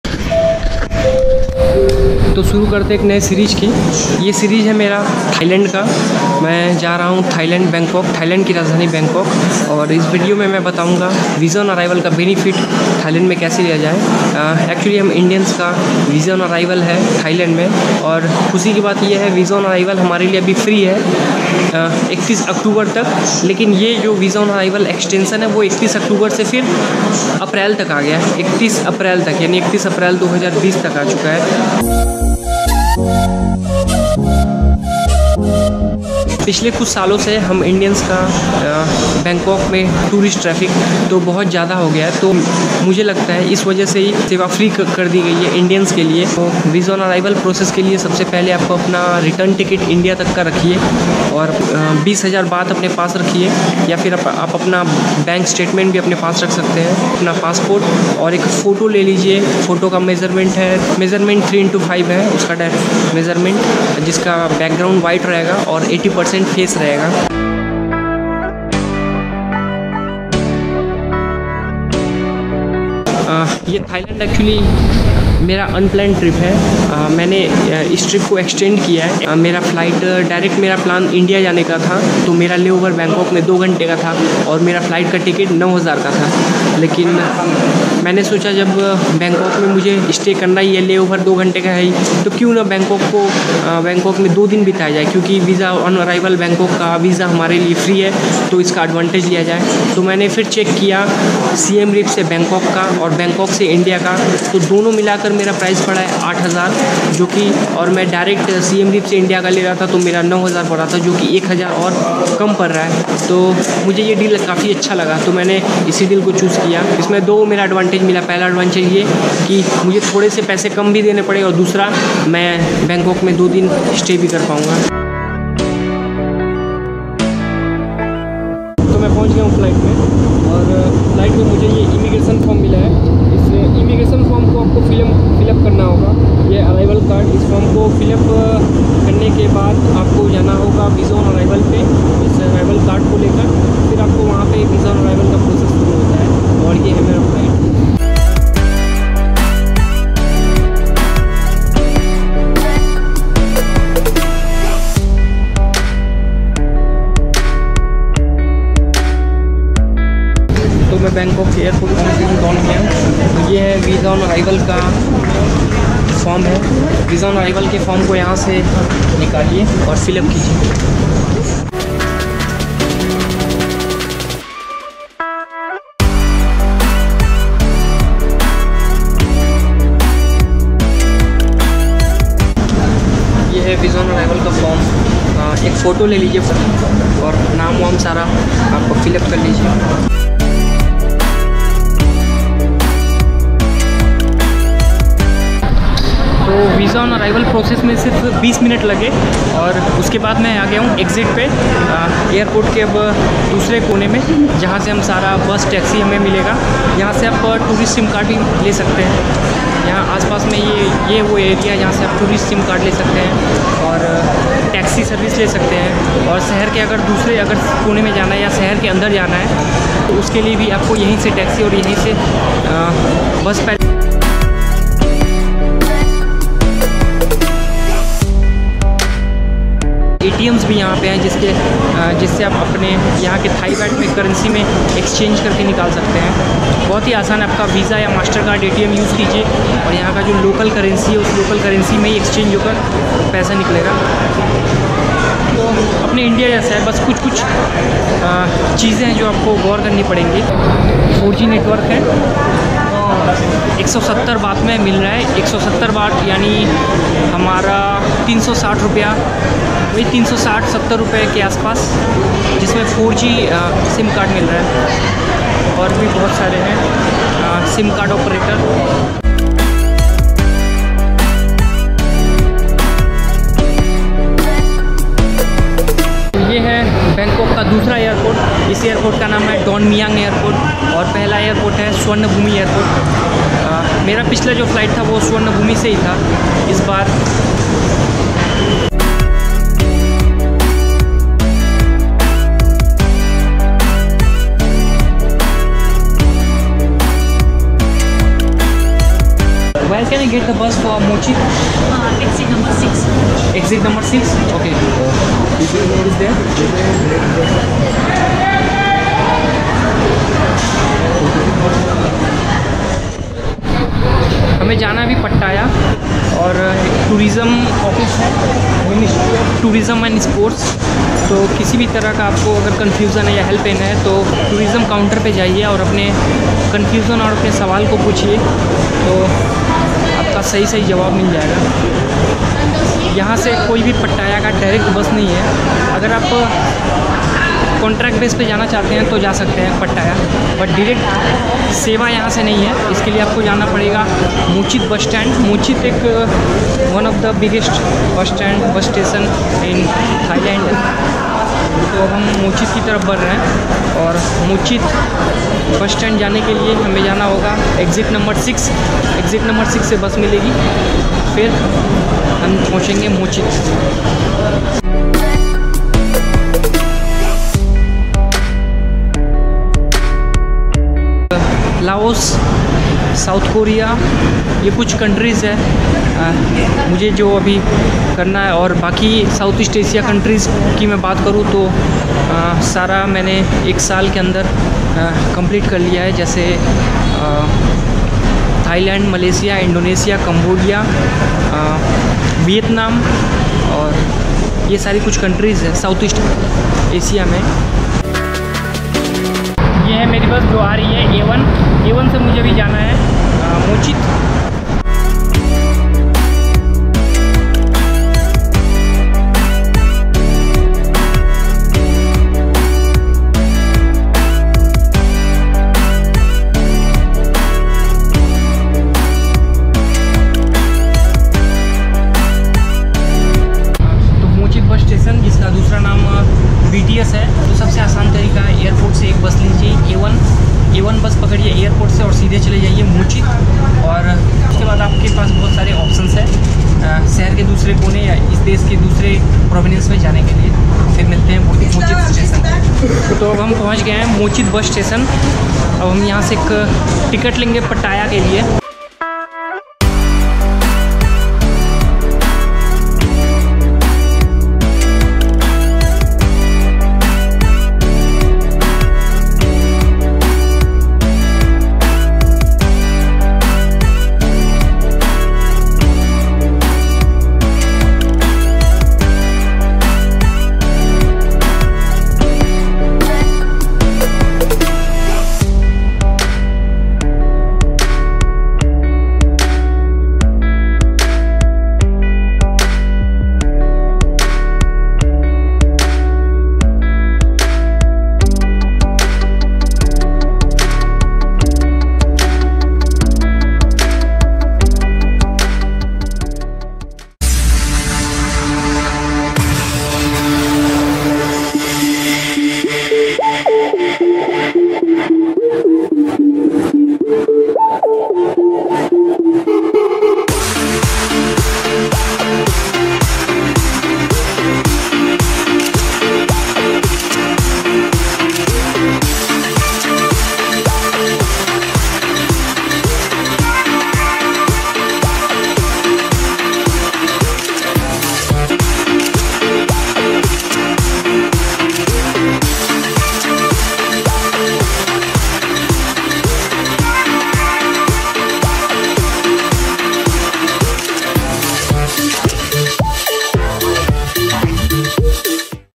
Cambodia to Bangkok Cheapest way | Thailand free visa on arrival for indians.
Cambodia to Bangkok | Thailand free visa on arrival for indians.I have shared everything starting from Visa on arrival of bangkok to mochit bus station and bus for pattaya.